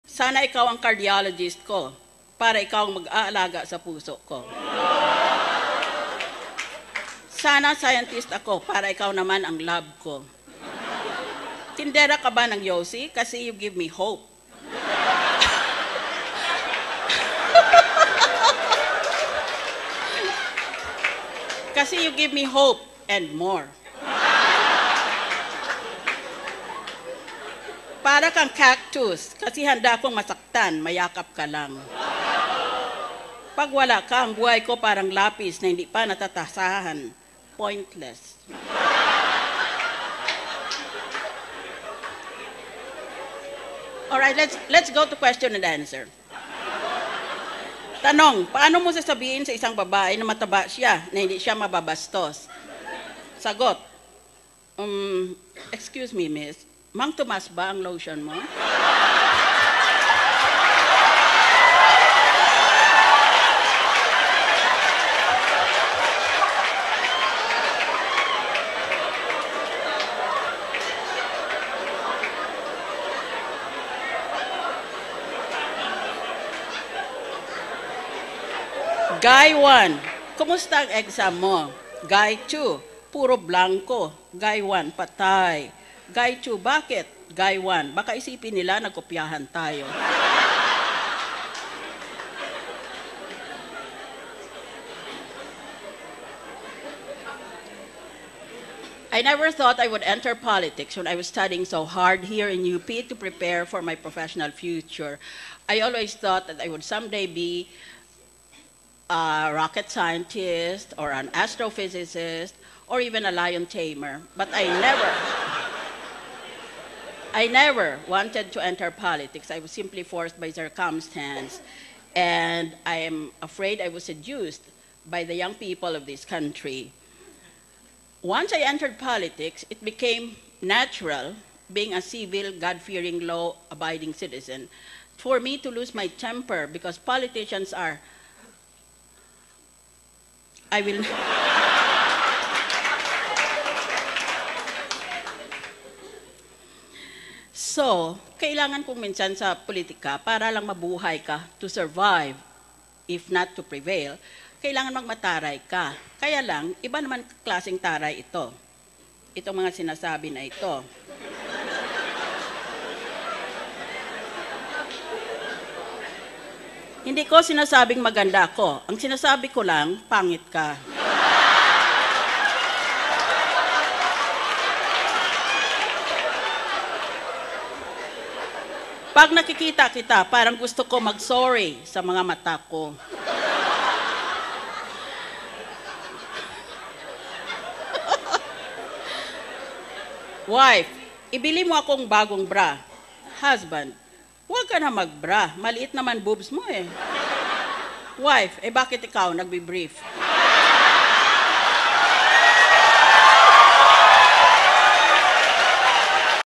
Part 3: PICK-UP LINES, ANECDOTES, LECTURE & MORE!
Kung si Senadora Miriam Defensor Santiago ang magbibigay ng lecture sa paaralan, gising na gising ka talaga! Ayaw mo ng matapos ang lecture. Simula pa lang, tawa ka na ng tawa! 🤣

This is part 3 of her very lively speech before students of the University of the Philippines. She gave a lecture about the RH Law.